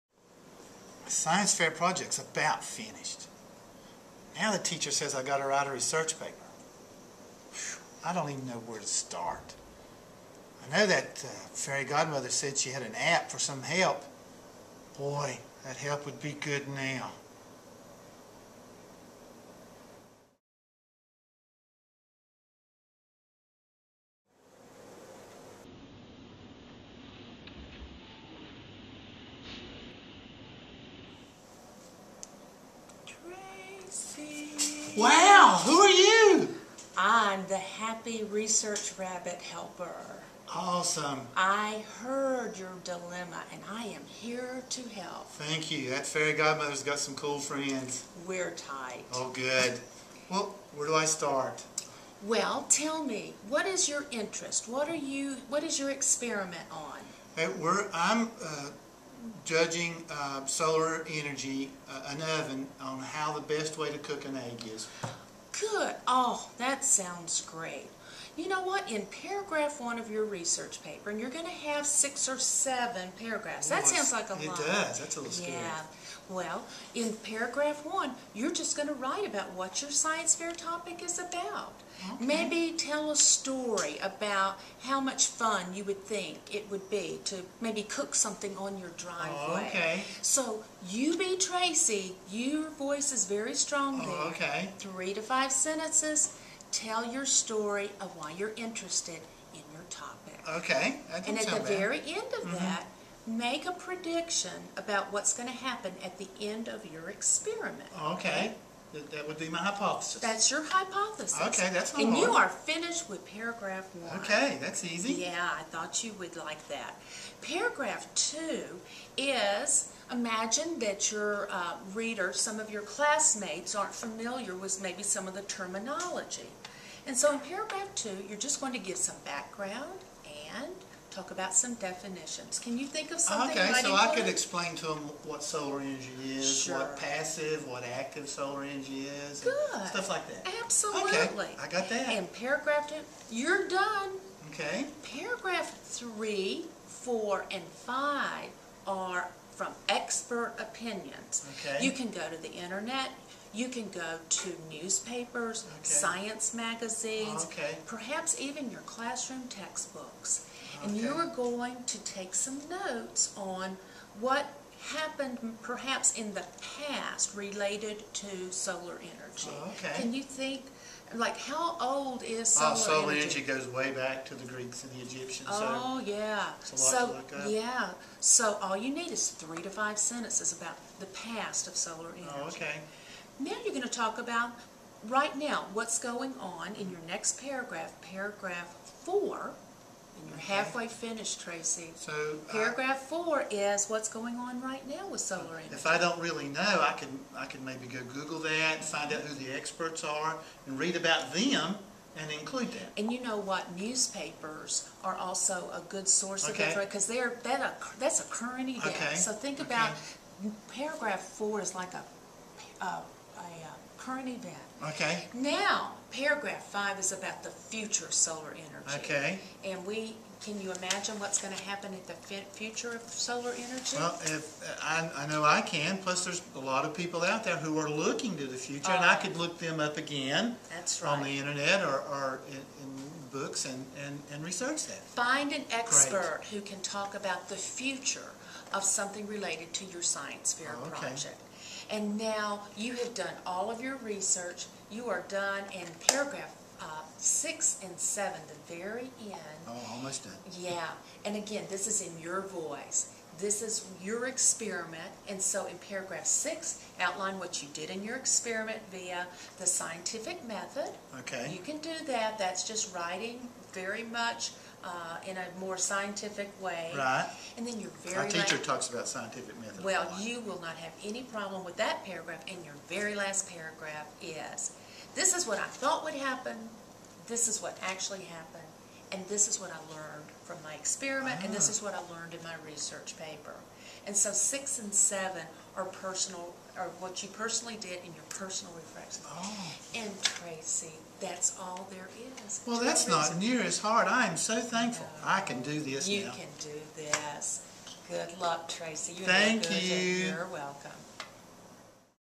Science Fair: Part 5 Abstract and Research Paper
Melanie Shaver and Tracy Childers in the fifth part of a six part series on how to do a Science Fair Project. Part 5 covers the Abstract and Research Paper